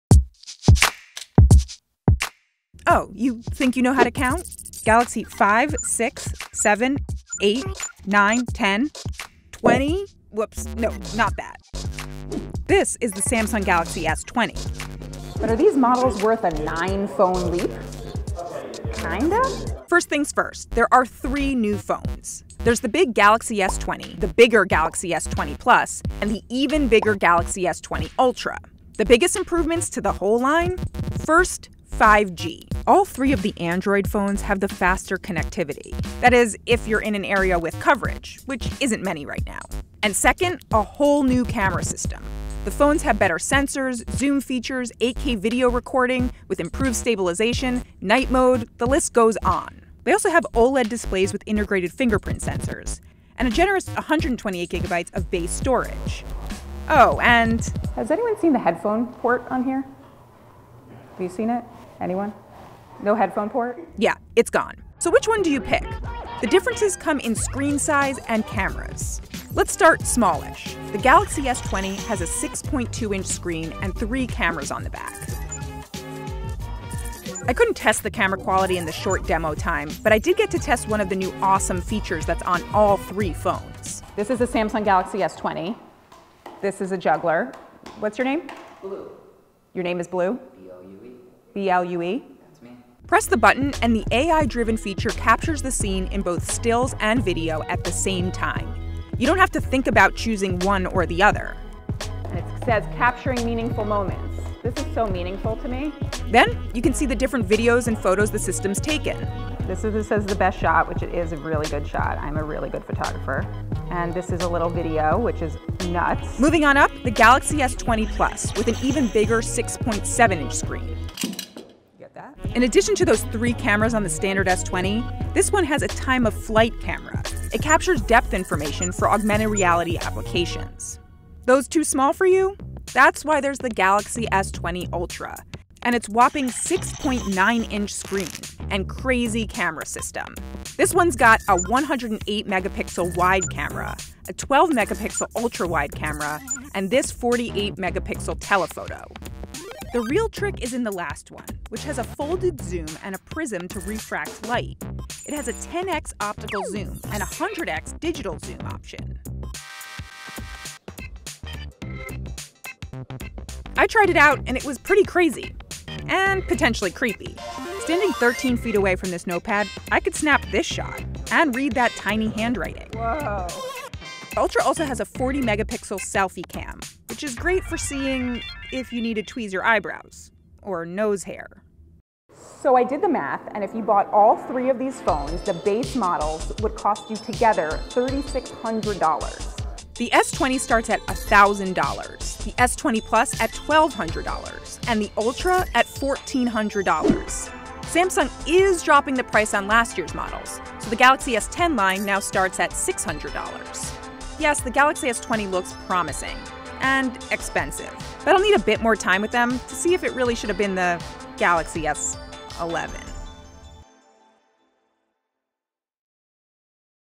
Samsung Galaxy S20 First Look: Three Phones, 5G, So Many Cameras | WSJ
This year's flagships from Samsung—the Galaxy S20, S20+, S20 Ultra—are packed. But can you tell them apart?  WSJ’s Joanna Stern explains how they differ in size, cameras, price and network connectivity.